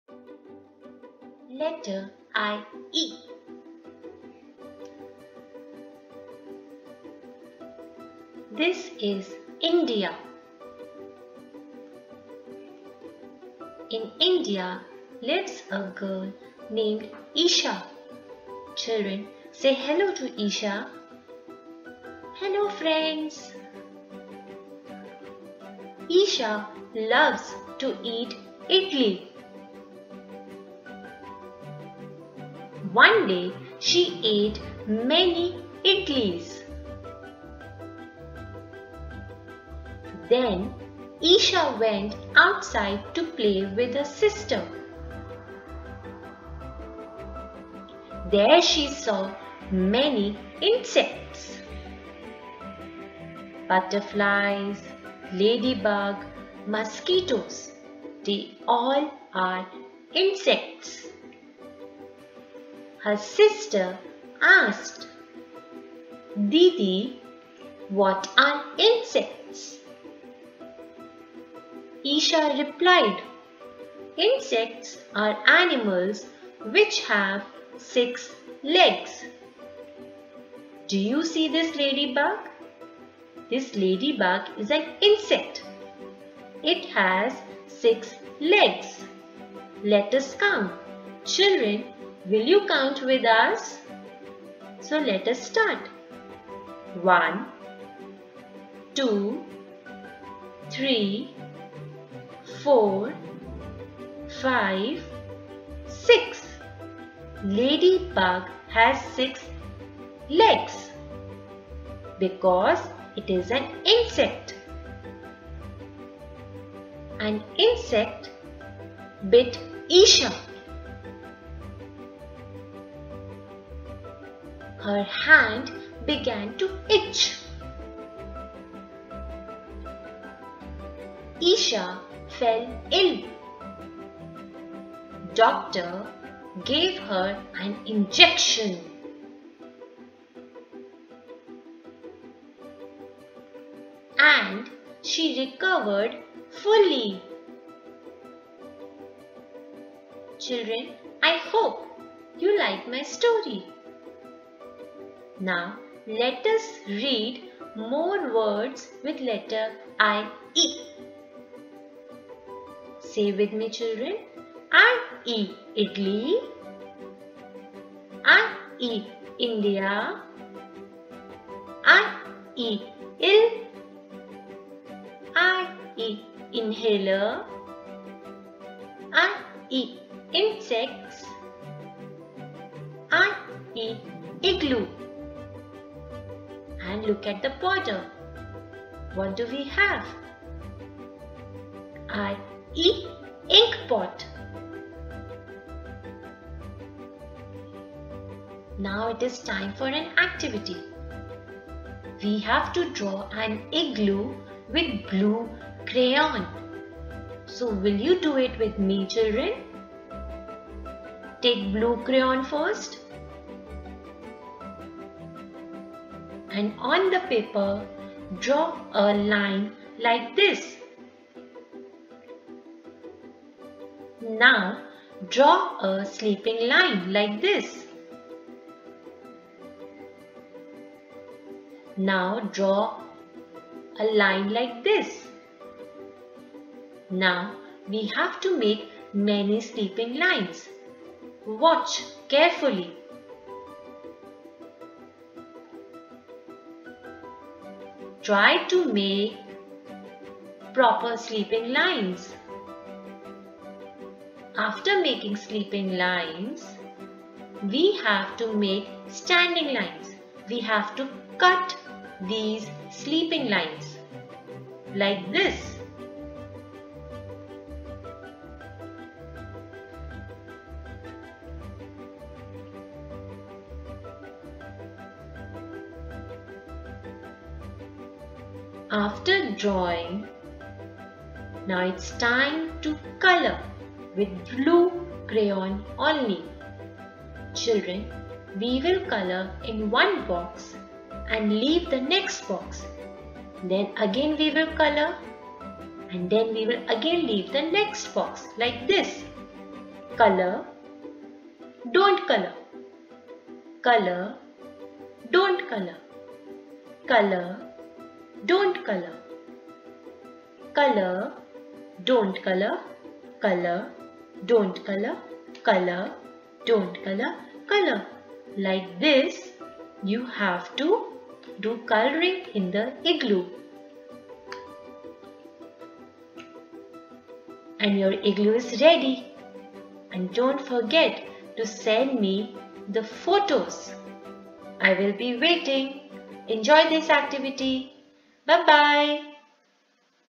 Letter ' I i ' with Charu Ma'am
Enjoy learning words with letter 'I i '. Watch till the end for letter 'I i ' activity.